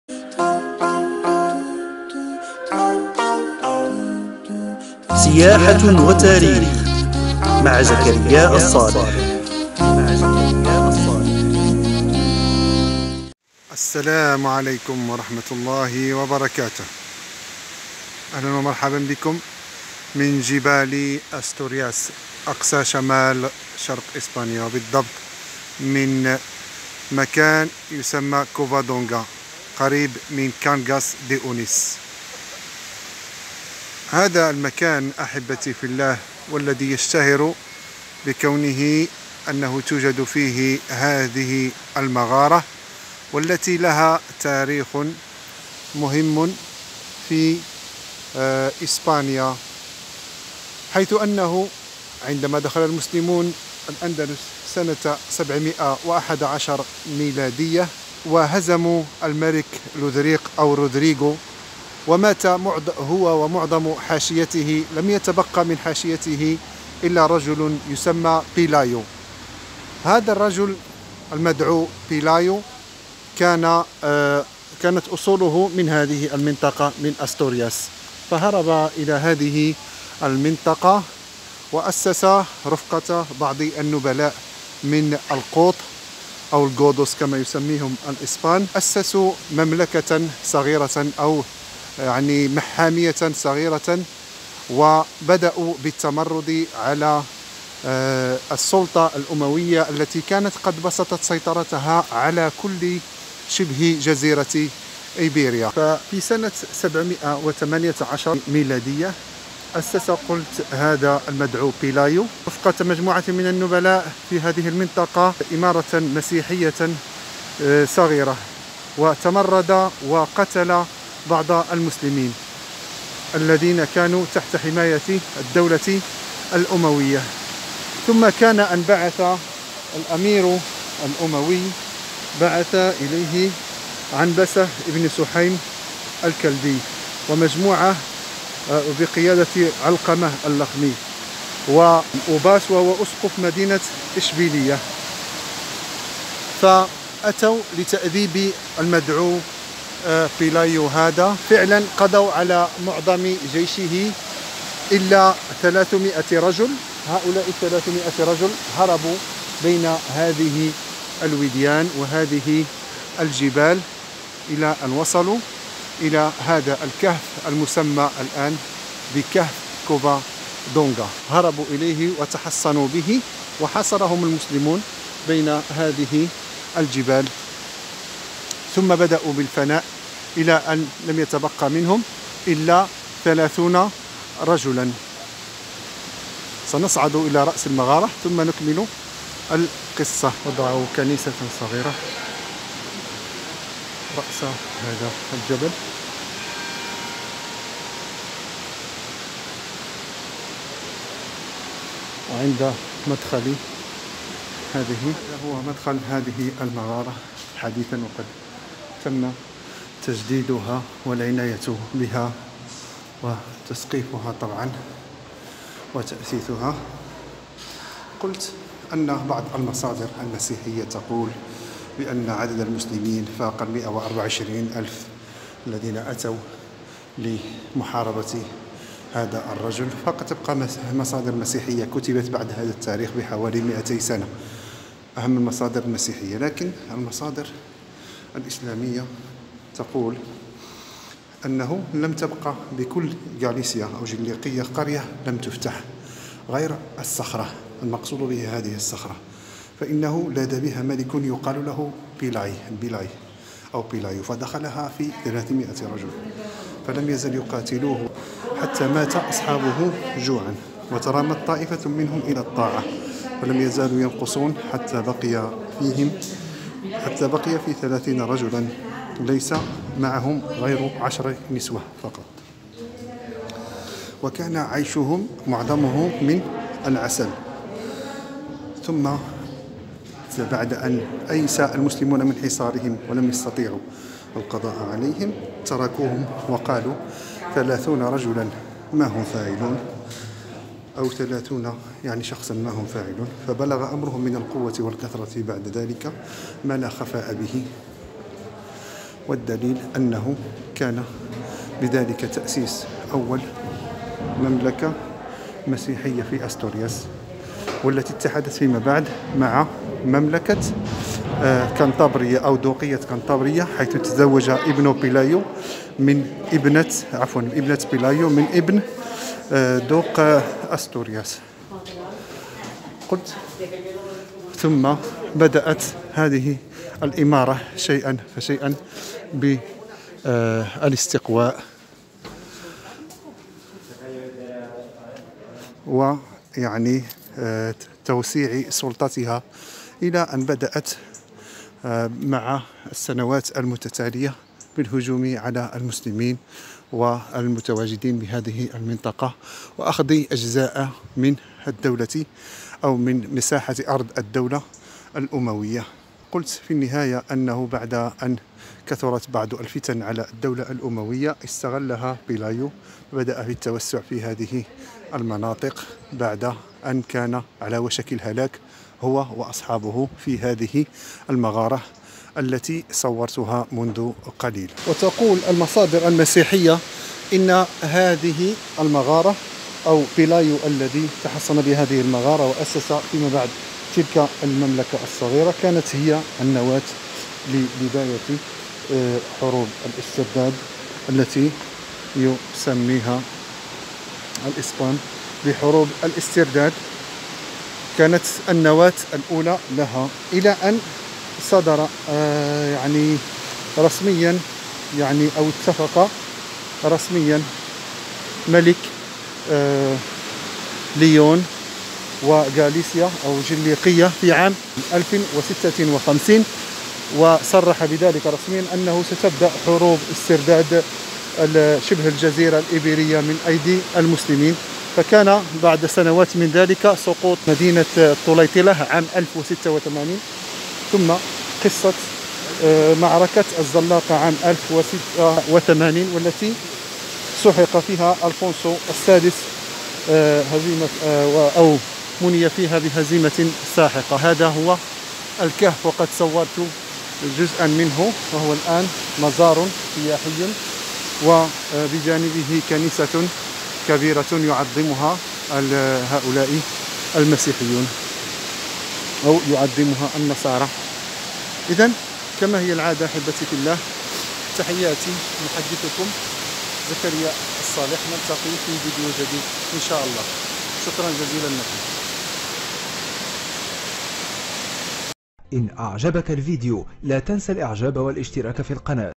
رحلات أندلسية 10 زيارتي لكهف كوفادونغا covadonga الذي كان سببا منذ سنة 721 ميلادية في سقوط الأندلس
يقع هذا الكهف في أقصى شمال إسبانيا ،وهو الكهف الذي تحصن به الملك pelayo   رفقة بعض حاشيته هربا من جيوش الفتح الإسلامي إلى أن مل المسلمون من حصاره وتركوه ..فارتكبوا بذلك أكبر خطأ استراتيجي تاريخي بالأندلس ..لأن تلك المجموعة كانت نواة لتأسيس أول إمارة مسيحية أقصى شمال الأندلس.